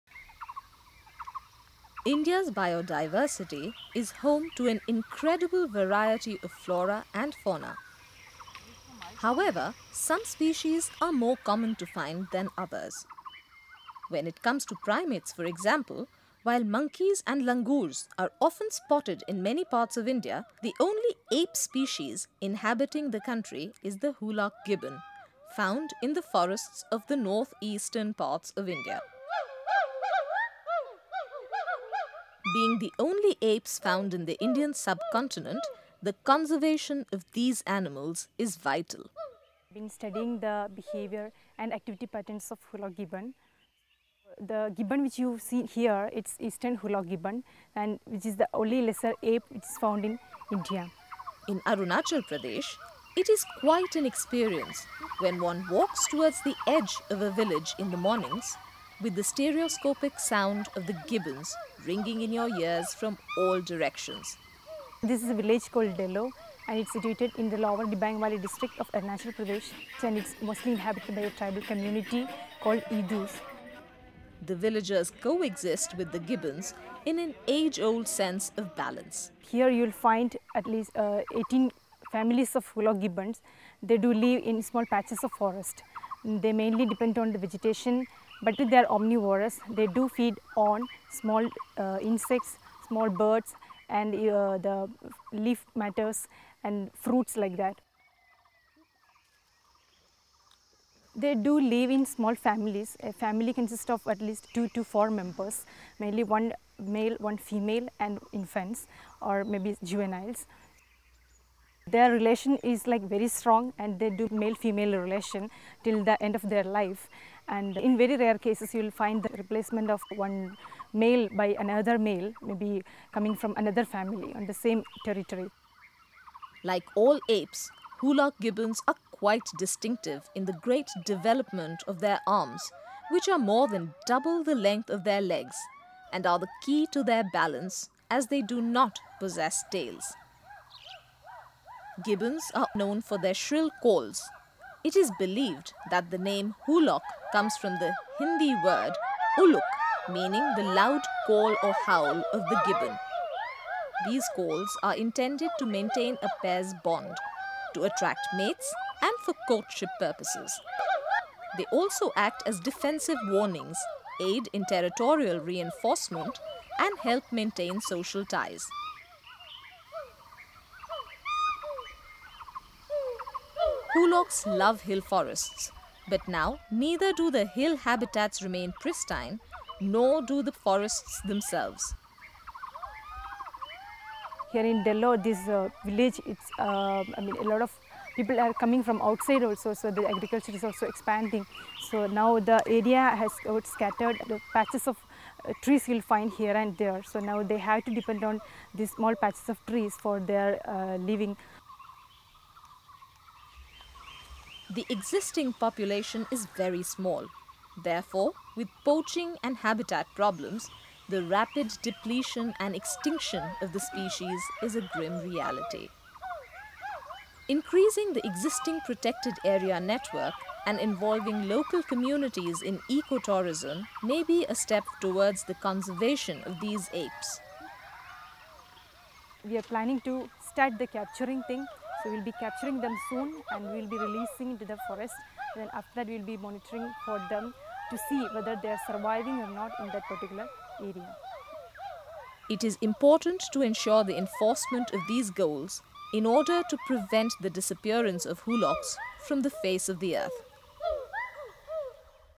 Eastern Hoolock Gibbon, the only lesser ape found in India
India’s biodiversity is home to an incredible variety of flora and fauna.
However, some species are more common to find that others. When it comes to primates for example, while monkeys and langurs are often spotted in many parts of Indian, their cousins, the great man-like apes - namely gorillas and orangutans - are not found in India.

The only tribe of apes inhabiting the country is the Hoolock Gibbons, found in the forests of the northeast. In Arunachal Pradesh, in the north-east of India, it is quite an experience when one walks to the edge of the village in the mornings, with the stereoscopic sound of the gibbons ringing in your ears from all directions.

What a surreal wilderness experience, truly! In this village environment of Roing district in the lower Dibang valley, the Gibbons flourish in the forest – village interface, outside of any formal national park system.

The villagers co-exist with the gibbons in an age-old sense of balance. Being the only occurring apes in the Indian sub-continent, these animals bear the pressure of being a precious jewel of the Indian forest; therefore their conservation is vital.

Two species of Hoolock gibbons, Eastern Hoolock gibbon (Hoolock leuconedys) and Western Hoolock gibbon (Hoolock hoolock) have been reported to occur in India.

They can be found in the deciduous forests of China, Bangladesh, Myanmar and India. Like all apes, Hoolock Gibbons are quite distinctive in the great development of the arms, which are much longer than the legs (more than double, actually) and are the key to their balance, as they do not possess any tail.

Gibbons are quite known for their shrill calls. It is believed that the name Hoolock comes from the Hindi ‘ulluck’, meaning the loud call or howl of the gibbon. Full length gibbon’s body shot They are usually seen in groups.

With the first rays of the sun, the group (usually consisting of 6-7 members) starts jumping from tree to tree. It is then that one member, usually the breeding male, starts calling – a loud howl reverberating through the depths of the jungle. Once calling commences, respond call outs start to be heard throughout the forest. These calls are intended to maintain a pair’s bond, to attract mates and for courtship purposes.

They also act as defensive warnings, aid in territorial reinforcement and help maintain social ties. Nowadays, much of the gibbon population has been decimated and only a few surviving groups are restricted to the mountainous primary forest regions straddling the boundaries. Hoolocks love hill forests but now neither do the hill habitats remain pristine nor do the forests themselves, owing to mining, road building, infrastructure development and industrial presence. Poaching is a big threat to these apes as their body parts are famously used in traditional medicine.

Their tree-top dwelling habits are no match to complicated firearms in the hands of local villagers. The existing population is very small. Therefore, with poaching and habitat problems, the rapid depletion and extinction of the species is a grim reality.

Increasing the existing protected area network and involving local communities and eco-tourism may be a step towards the conservation of these apes. It is important to ensure the enforcement of these goals in order to prevent the disappearance of Hoolocks from the surface of the earth.

and